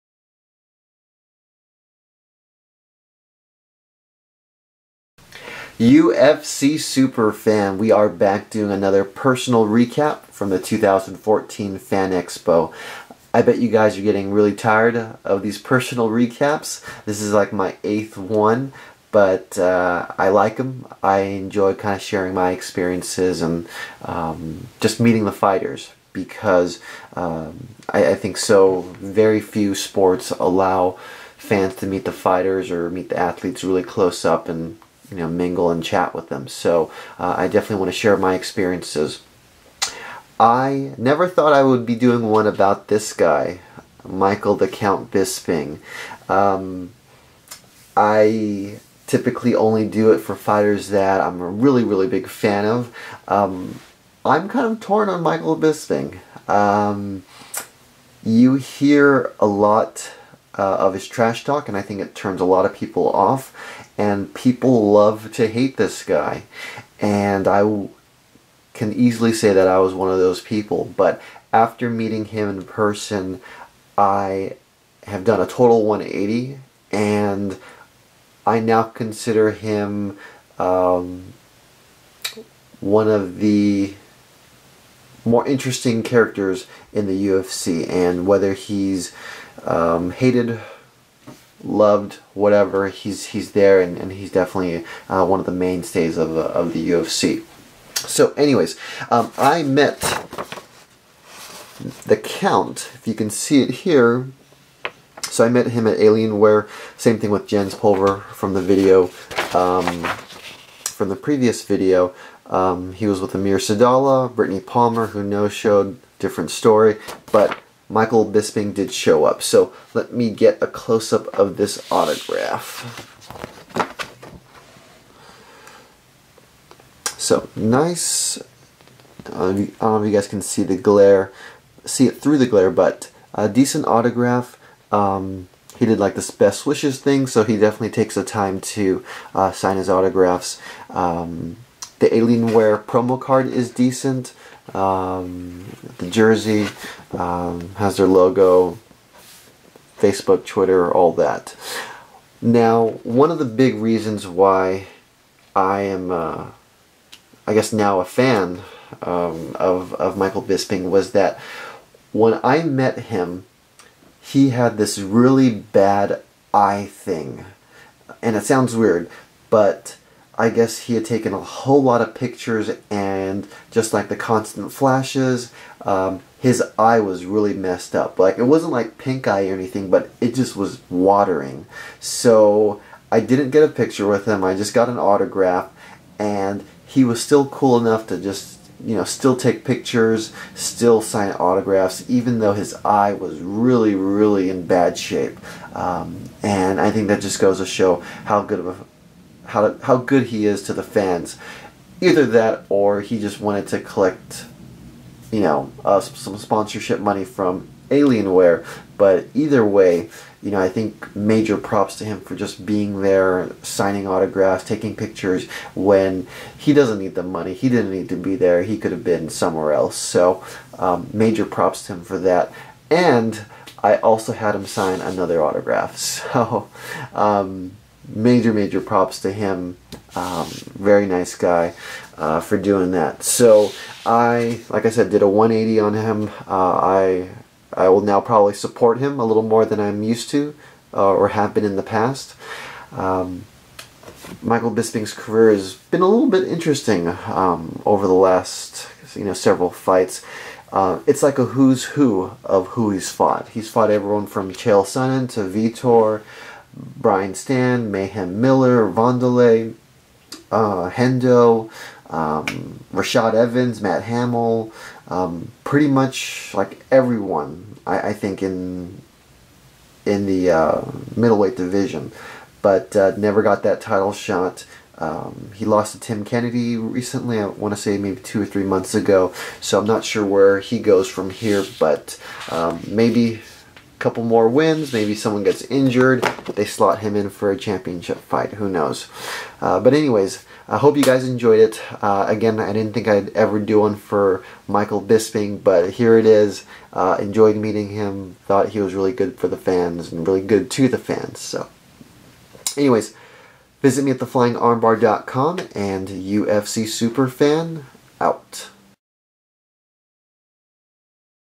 Michael Bisping - My Experience Meeting Him at the 2014 UFC Fan Expo and Getting His Autograph
I never thought I would be showcasing anything Michael Bisping.  I wasn't a fan of him and was trying to meet Brittany Palmer and Jens Pulver.  BUT I met him and glad I did.  He was battling an eye issue and looked pretty uncomfortable, but was taking pictures, signing autographs and joking around with everyone.

I still don't care of his trash talk and antics, but to the fans, he's great!